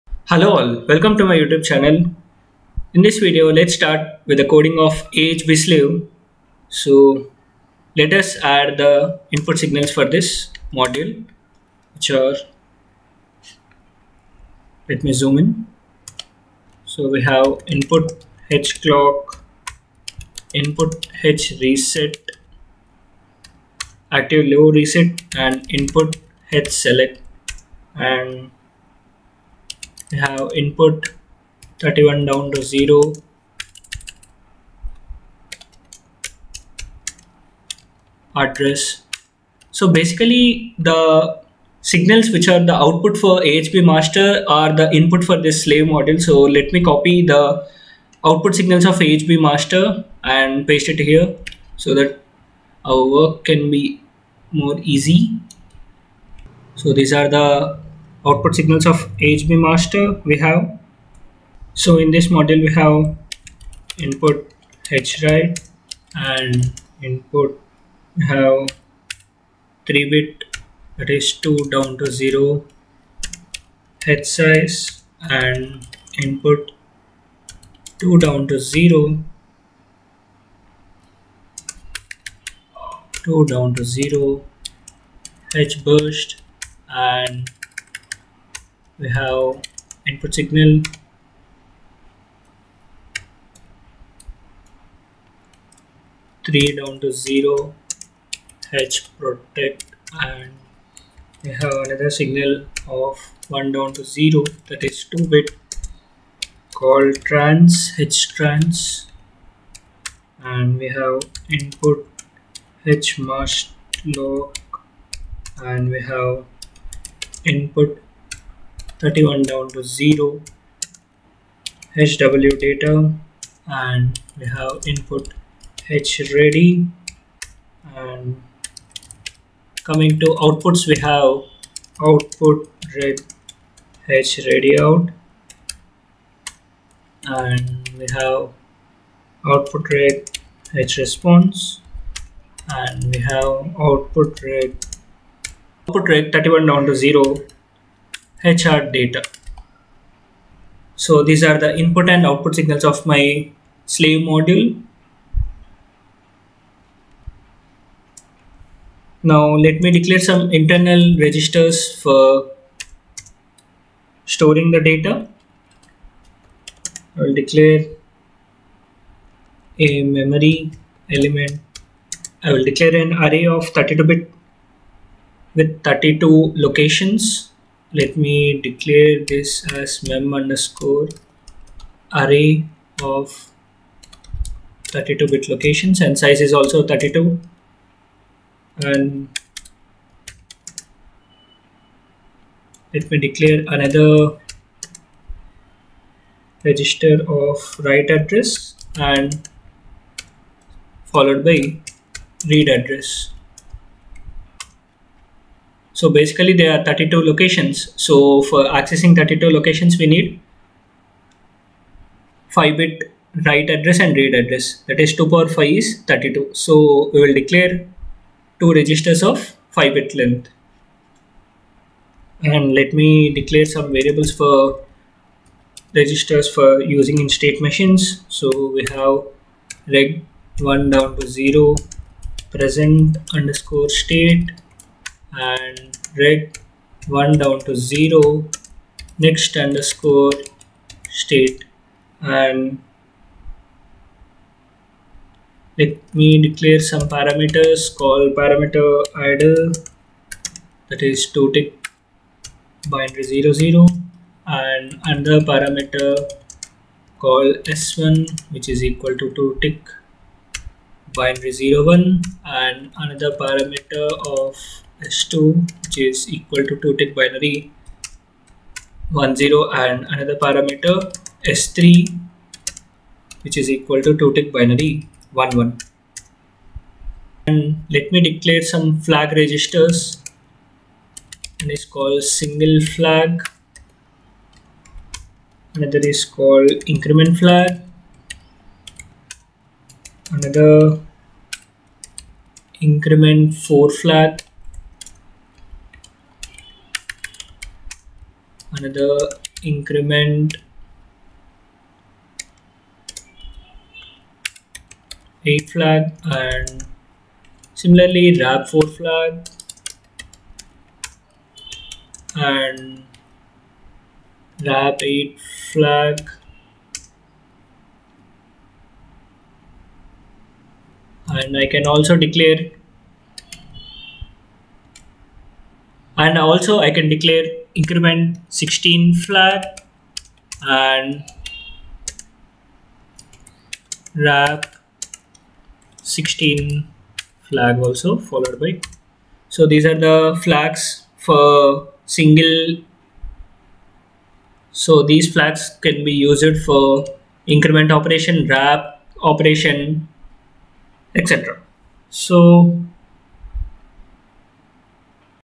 Verilog code for Ahb slave part - 1 || Amba Ahb Bus protocol
From this video I am starting with the verilog code for ahb slave of amba ahb bus.